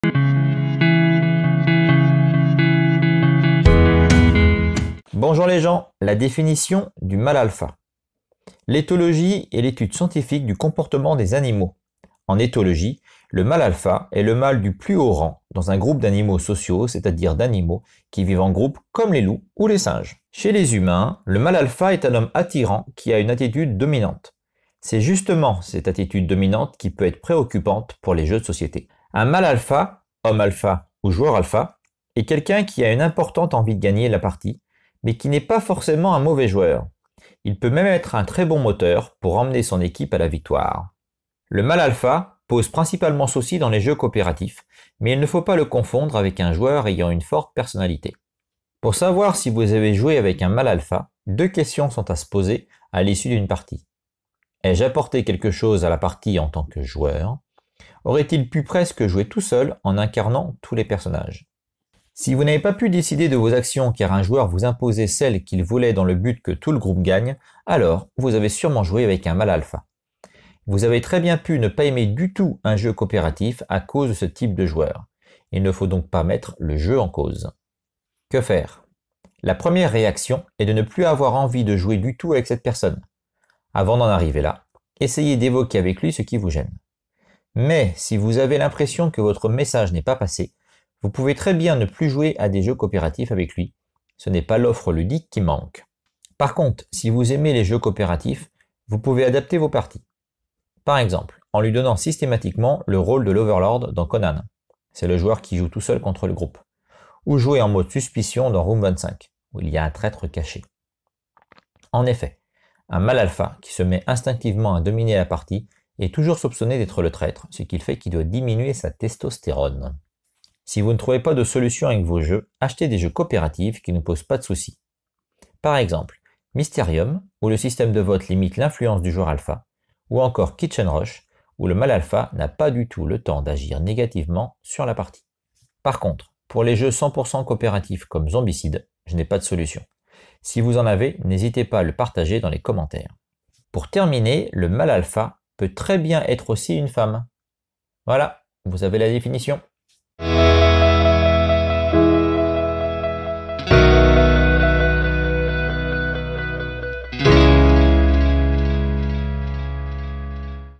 Joueur alpha : définition ludique
Explication en vidéo de la définition du mâle alpha (ou joueur alpha ou effet leader) dans le monde du jeu de société

⮮ 🛈 Plus d'informations 🛈 ⮯
🎞 Détail de cette vidéo 🎞

00:00 Avant-propos
00:08 Définition du mâle alpha
00:52 Avez-vous déjà joué avec un homme alpha ?
01:22 Que faire avec un joueur alpha ?
02:27 Conclusion